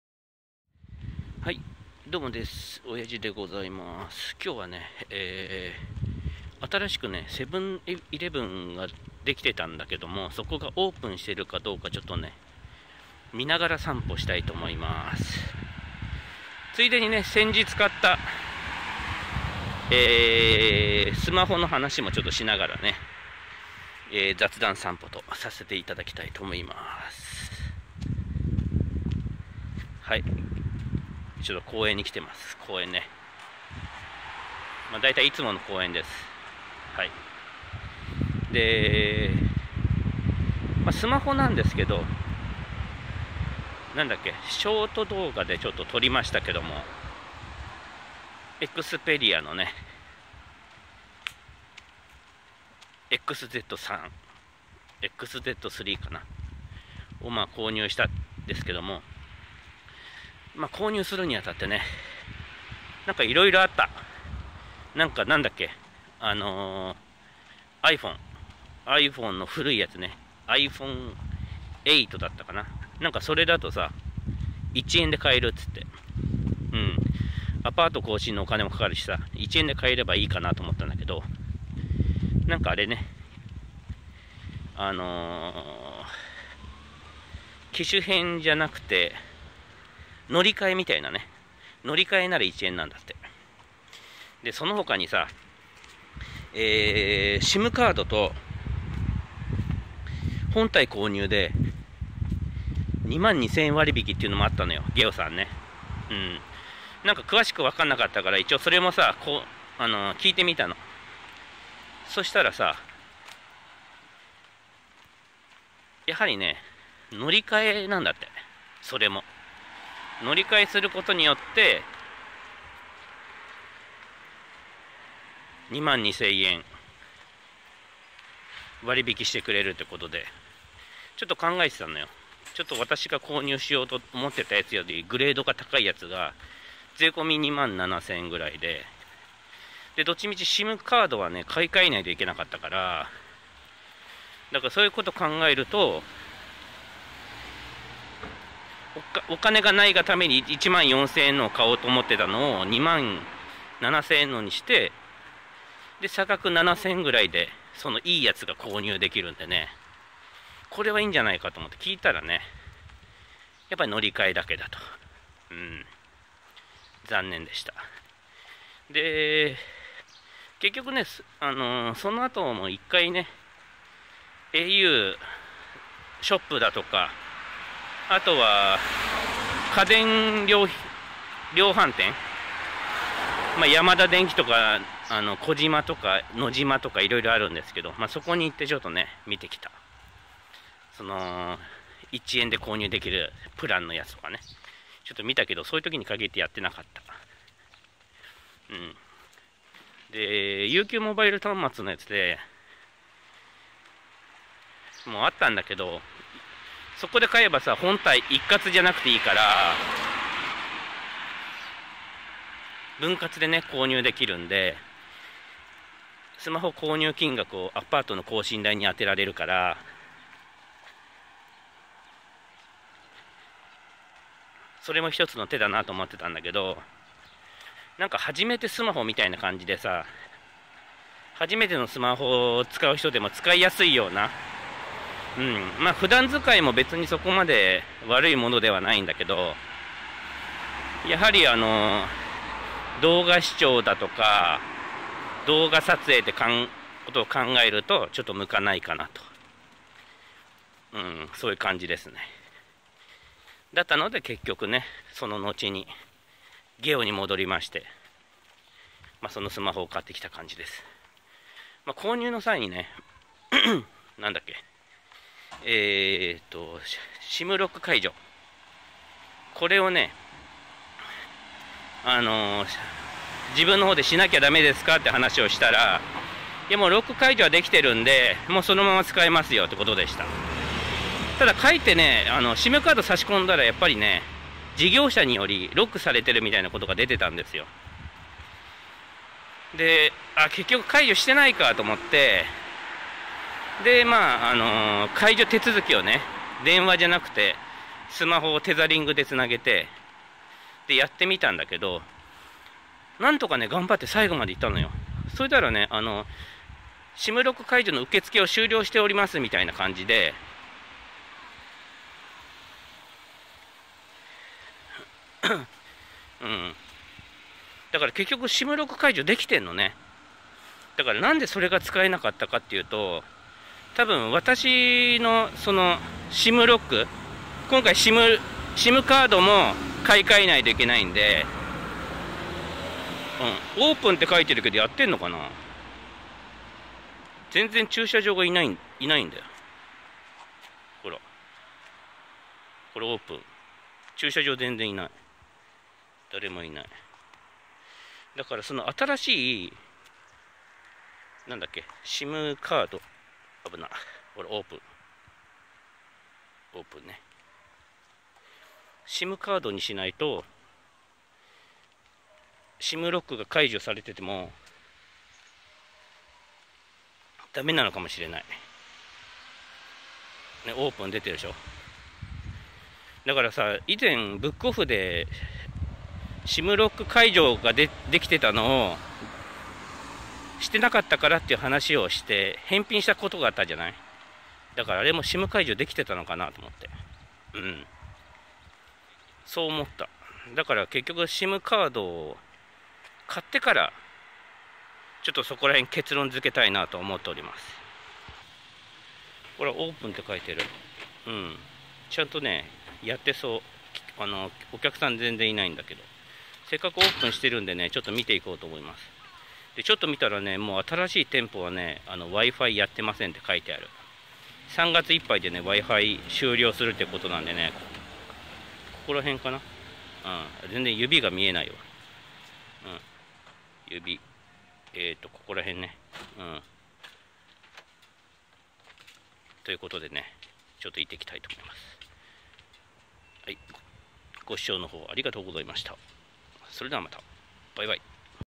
【セブンイレブンがオープンしたか見に行く❗】天気も良いしこの間購入したスマホの話しもしながら街中散歩📱
散歩はよい🎶